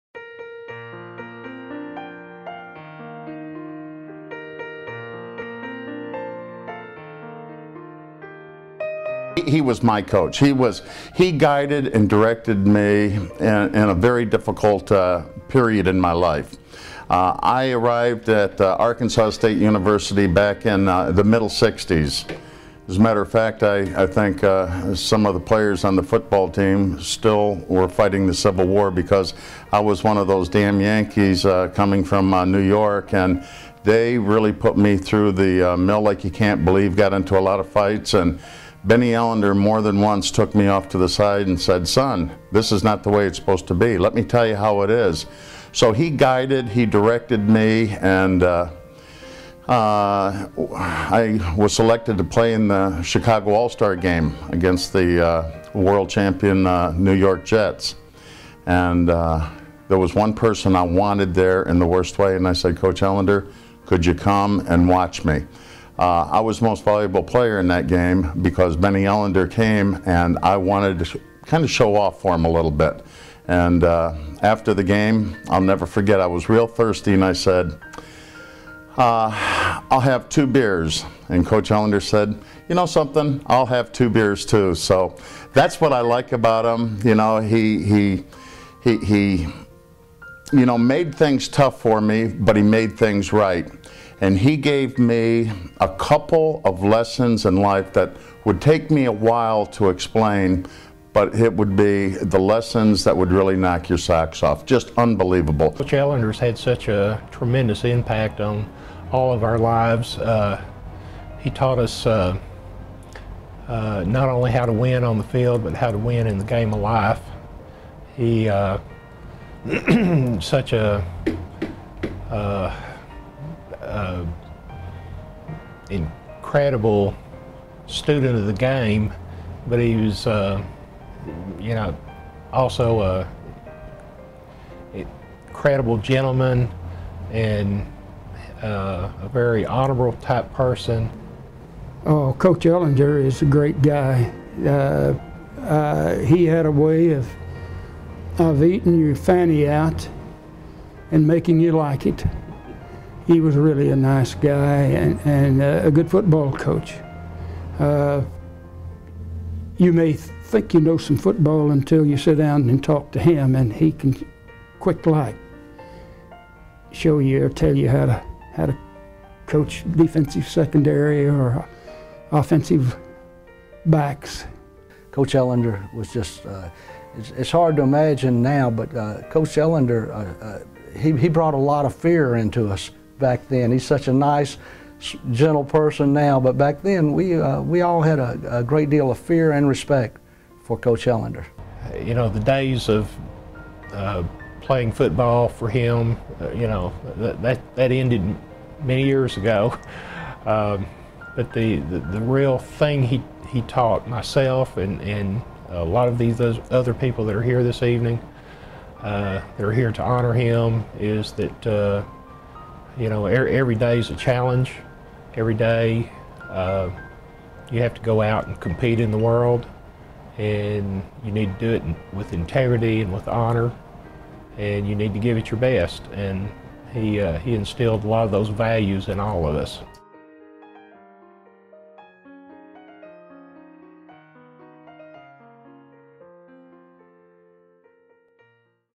Bennie Ellender Tribute
Former players and coaches share their thoughts on the legendary football coach.